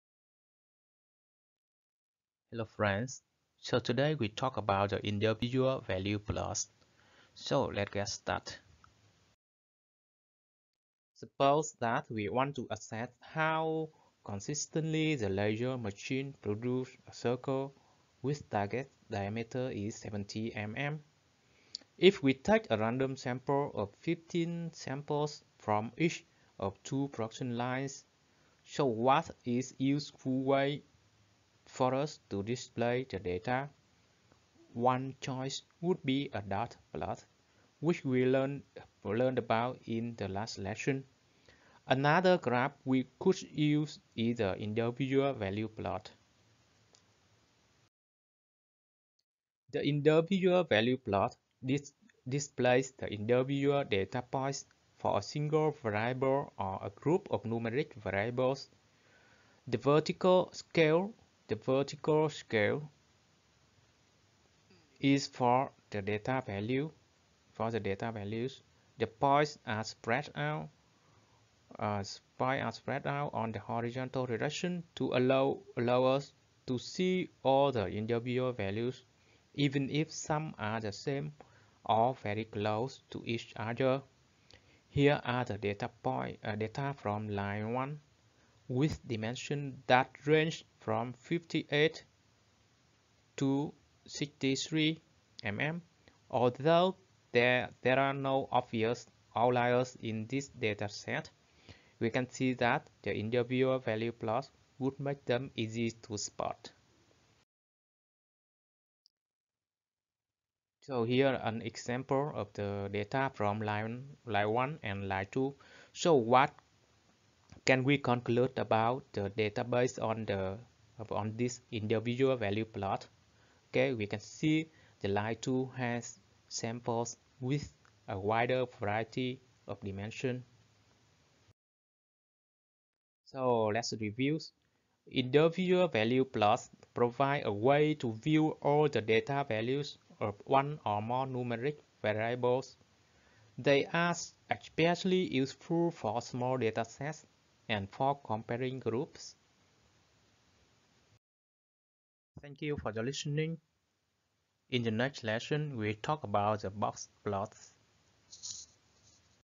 006 Individual Value Plots |Lean Six Sigma Training| Black Belt
Data visualization should not just show the averages, but also allow us to explore the outliers and variations within the data.